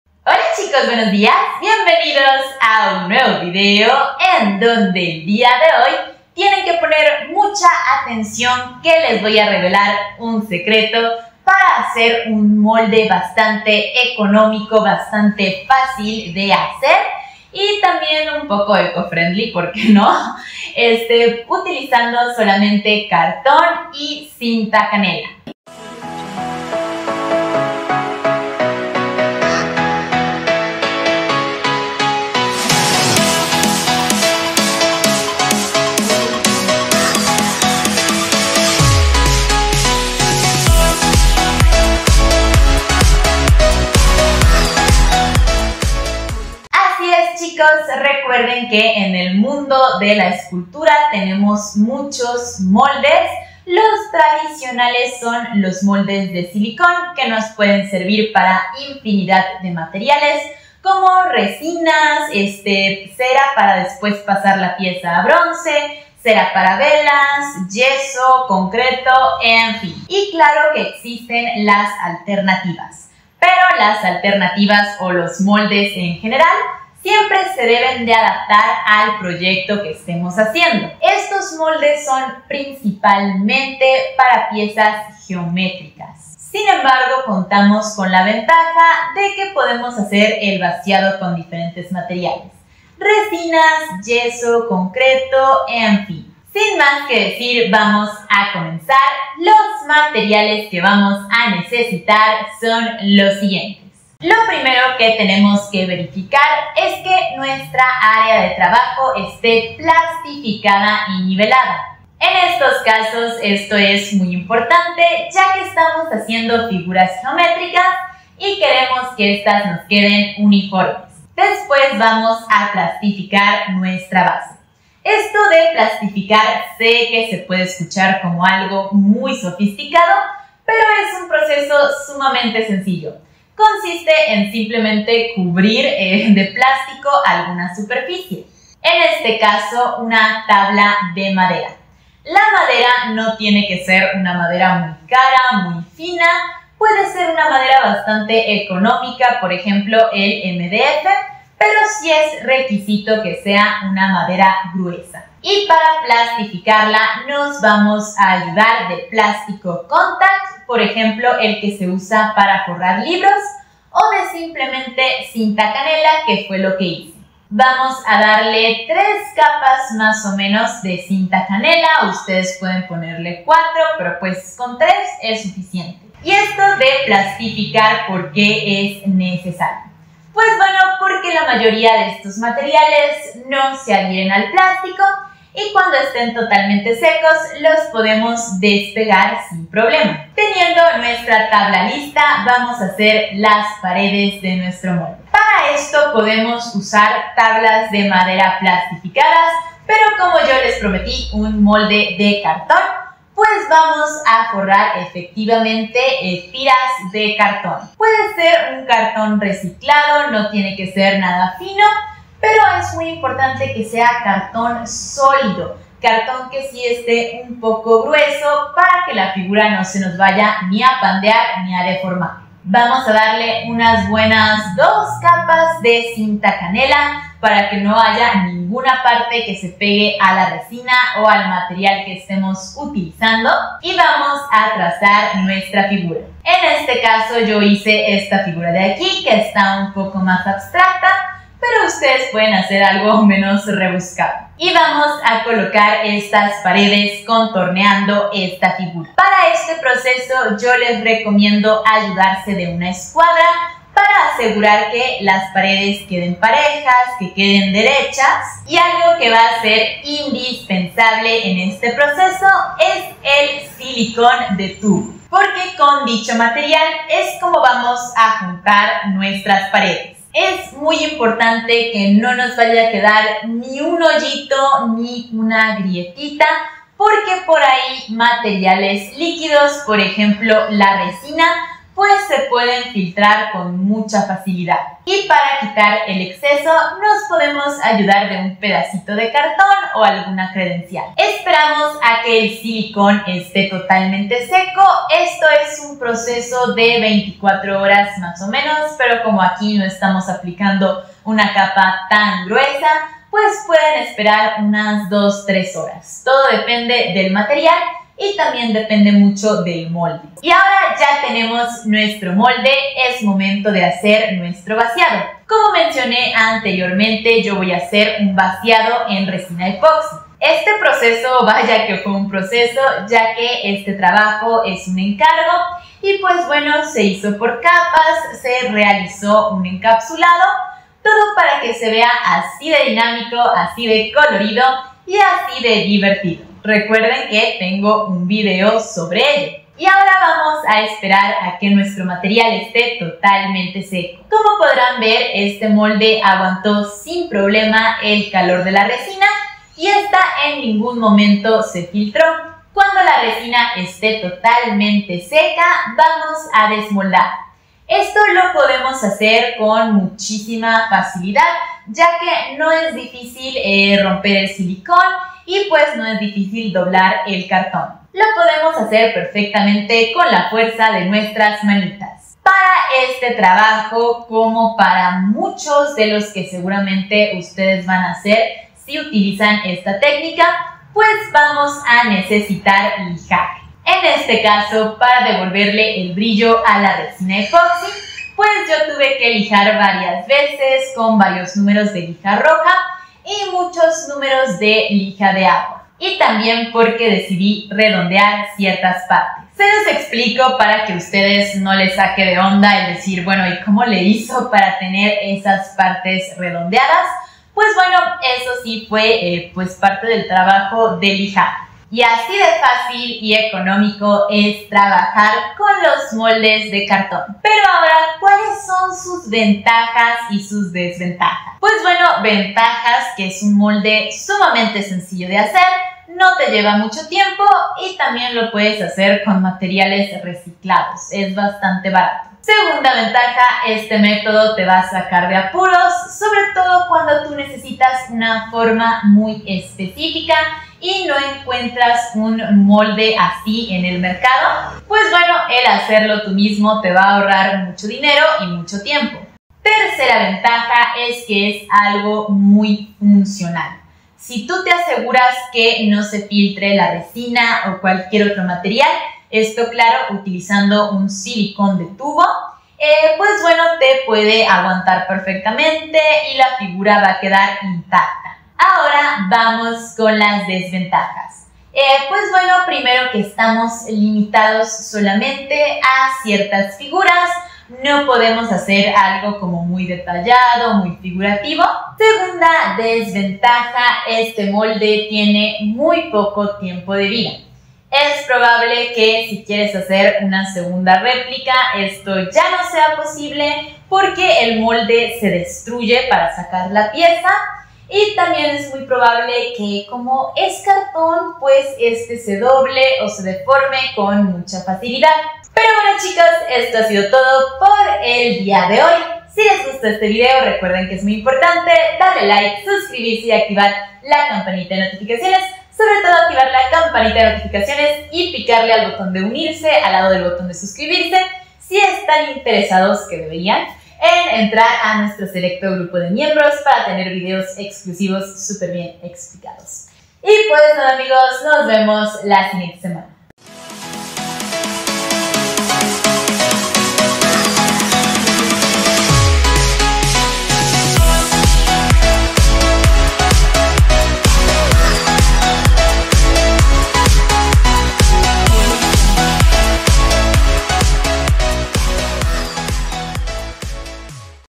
¿CÓMO HACER MOLDES DE CARTÓN? ¡BARATOS Y FÁCILES! para RESINA EPOXI, YESO, YESMOITA, ETC
Intro 00:00
Área de trabajo 01:44
Base 02:18
Paredes 03:10
Silicón de tubo 04:04
Vaciado 05:10
Desmoldar 05:52
Ventajas y desventajas 06:51
Resultados 08:25